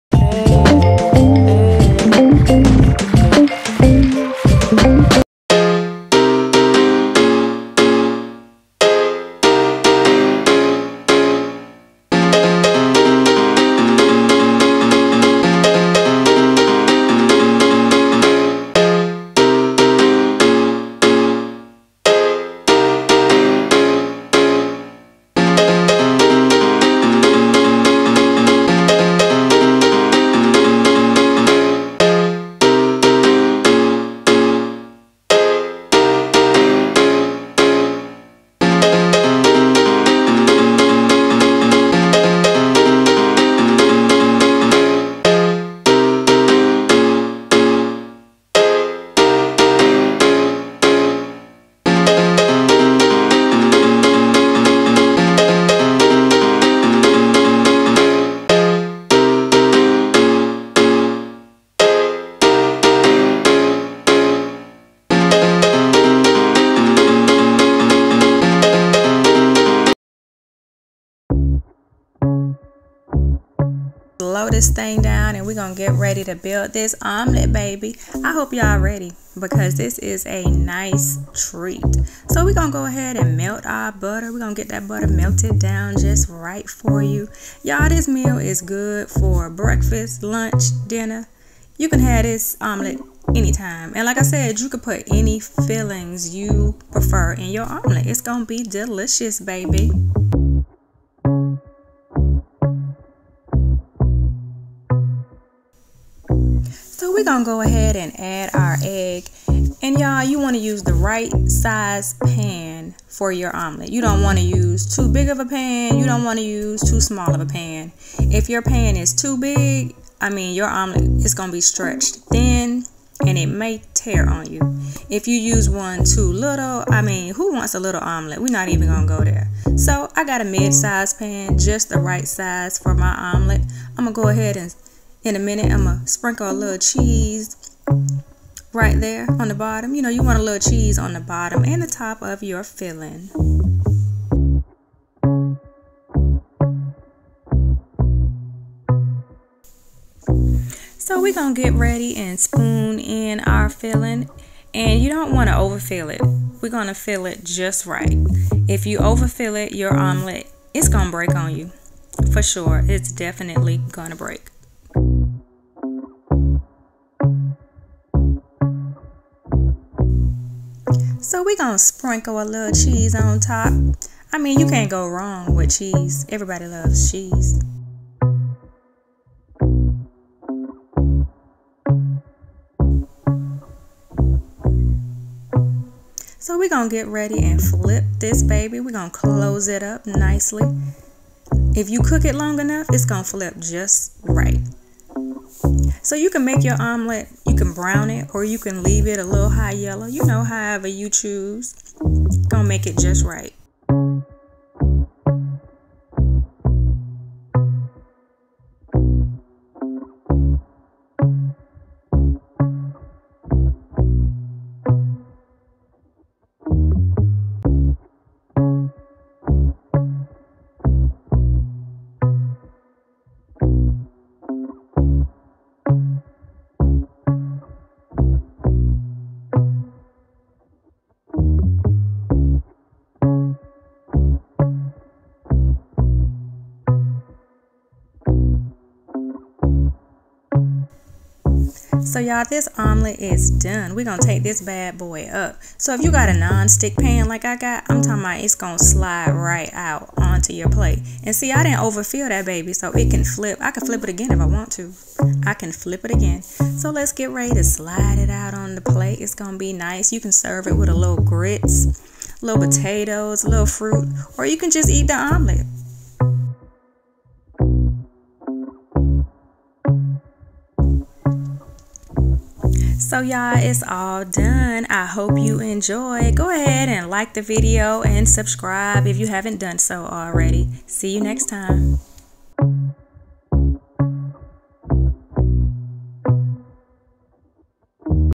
Lip Smacking Lobster, Scallop & Shrimp Omelet
Simple, quick method to making an omelet.  Left over seafood was used for this omelet.  Leftovers are perfect for making a quick omelet.  Leftovers don't need seasoning.  If you are using leftovers, saute them long enough in the butter to heat up. Saute any fresh ingredients in butter that you want to use to prepare your omelet.  Add seasoning to fresh ingredients.   I don't add seasonign to my egg.  Nothng was added to the egg.